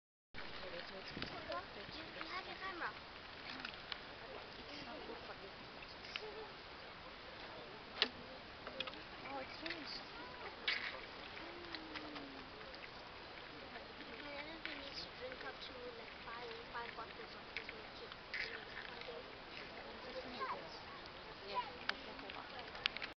Angerer Family adopted a baby Elephant named Lentili, Nairobi, Kenya 2014.
Angerer Family ( Roland,Wanny, Pavel & Elisa) adopted a baby Elephant named Lentili, at the David Sheldrick Wildlife Orphan Trust, Nairobi, Kenya. 2014.